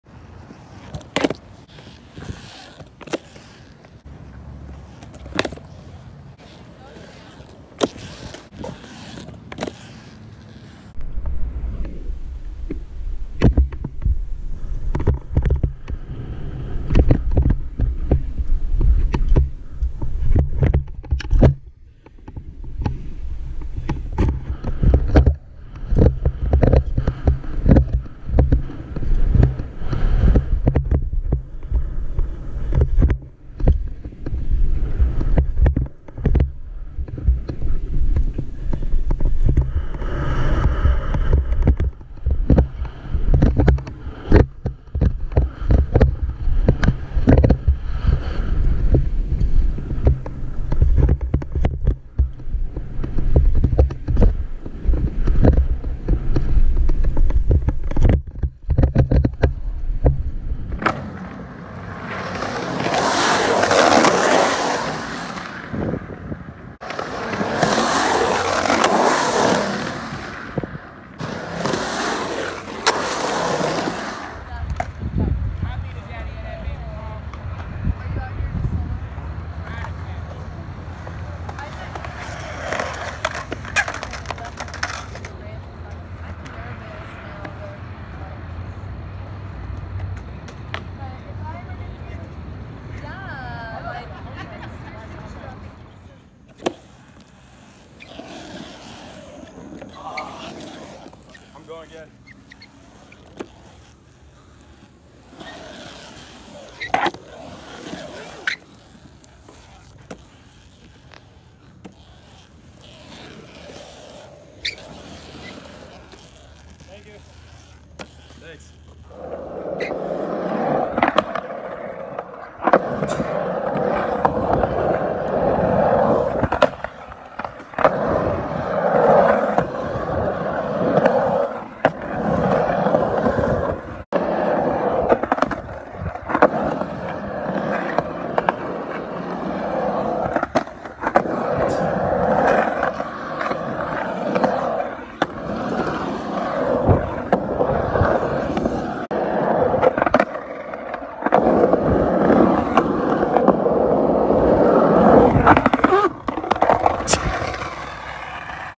Gathering Place Skate Park & Pump Track (Tulsa, OK)
Riding BMX and skateboarding at the gathering place skatepark and pump track in Tulsa Oklahoma around 2018